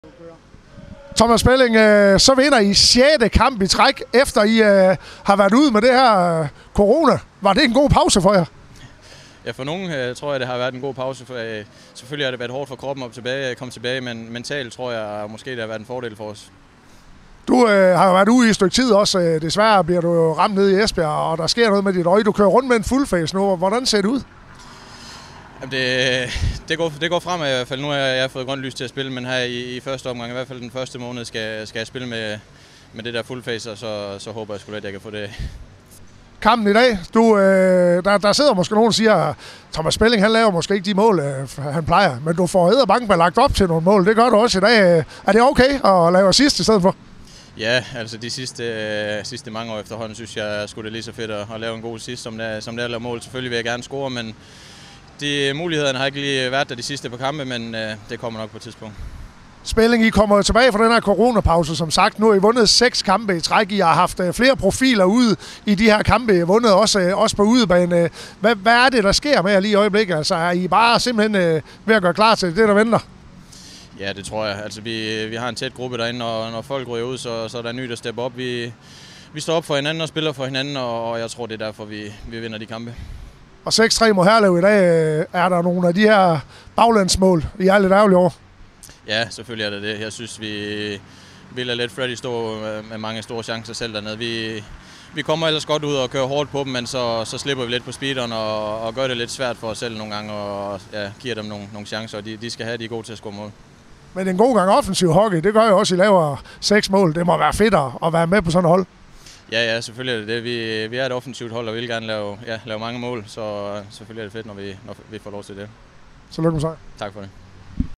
Aalborg Pirates-Herlev Eagles 5/2-2022.
Rishøjs Corner fangede Aalborg Pirates Thomas Spelling efter sejren på 6-3 over Herlev.